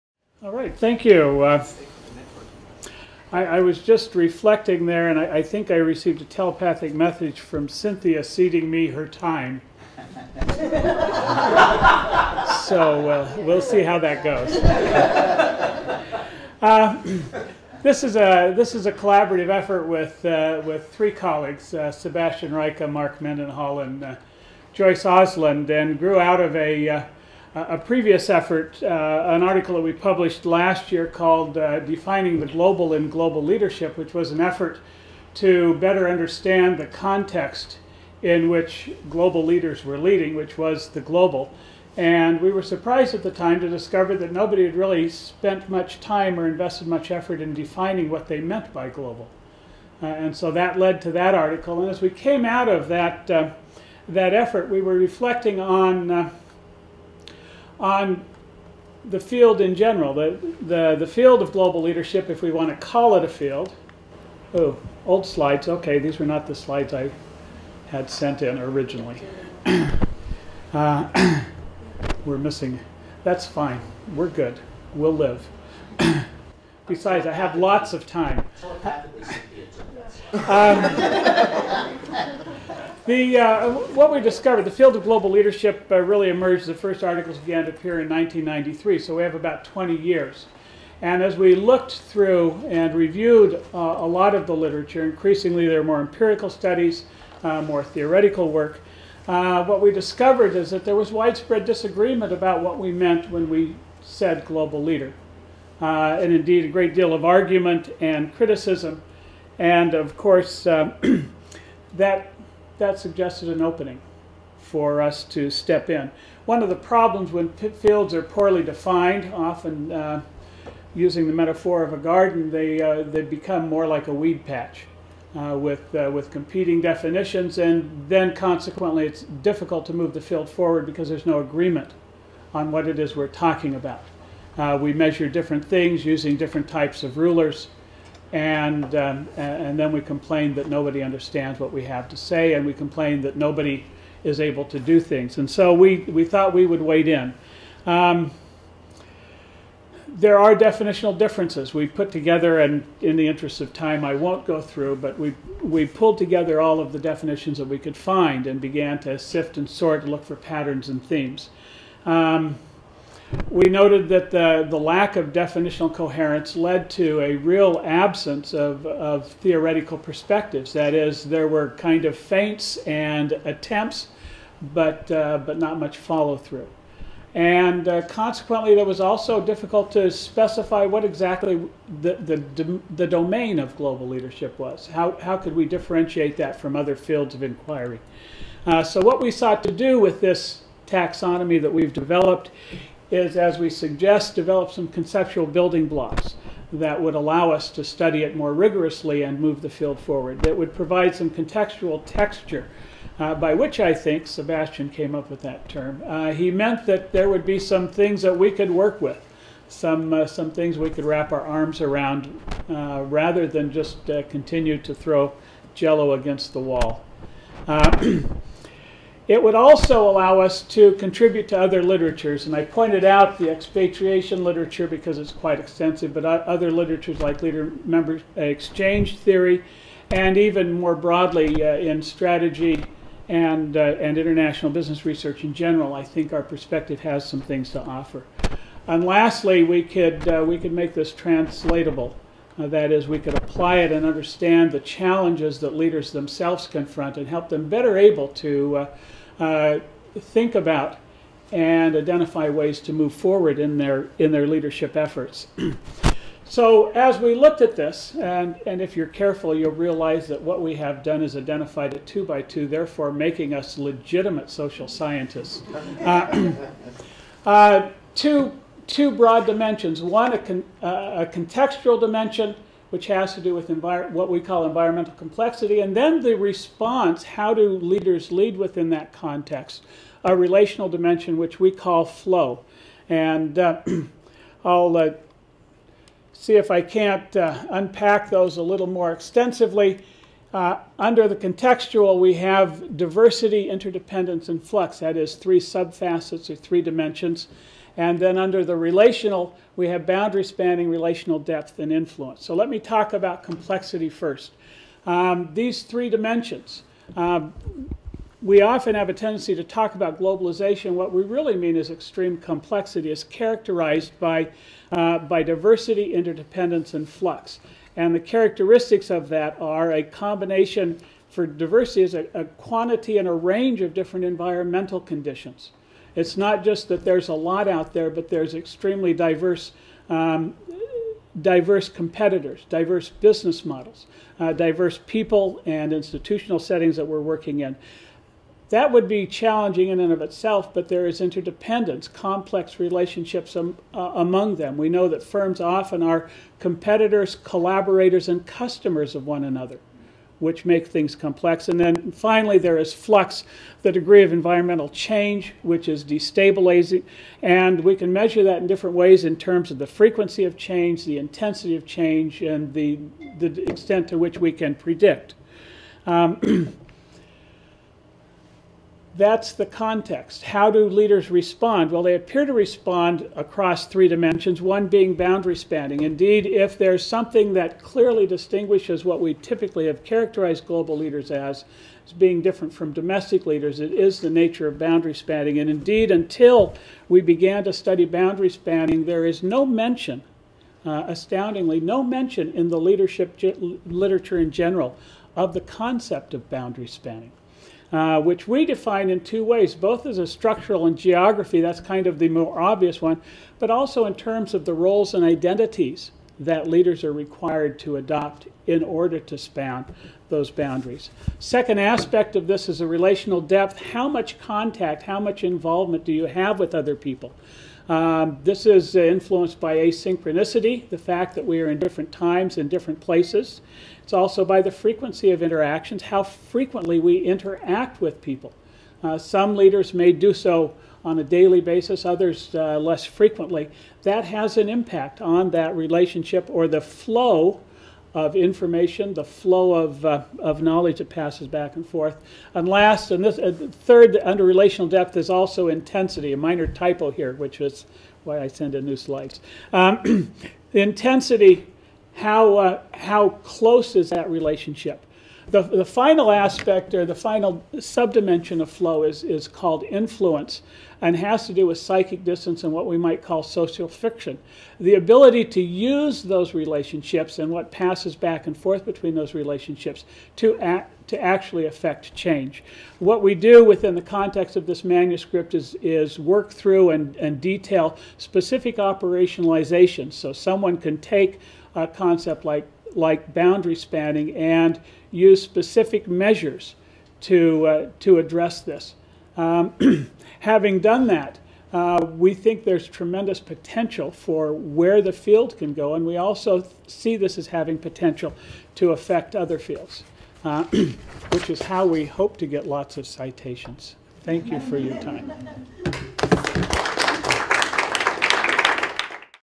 Allan Bird's Research Presentation at the D'Amore-McKim Research Showcase III
Watch Allan Bird*, Northeastern University, "Towards a Taxonomy of the Global Leadership Construct" presentation at the 2014 D'Amore-McKim School of Business Research Showcase III at Northeastern University, Boston MA.

*Additional contributors includes Mark Mendenhall, University of Tennessee -- Chattanooga and Joyce S. Osland, San José State University